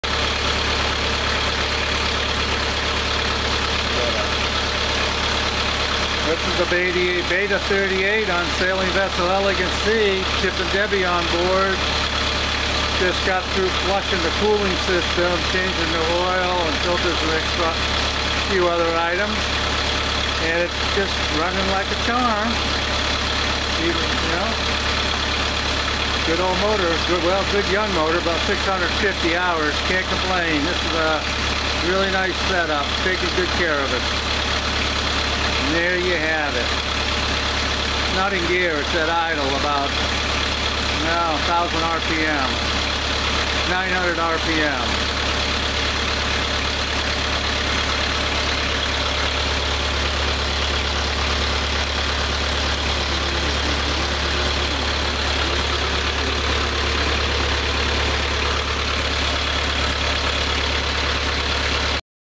s\v Elegant'sea-Beta 38 hp running at 650 hours - 2015
This is our Beta 38 hp with ~650 hours on it. We have just flushed the cooling system and changed the oil and we did some other maintenance. Getting ready for cruising the Mexican Riviera again this coming season. Our sailboat (s\v Elegant'sea ) is a  36' 1978 Islander Freeport. 2015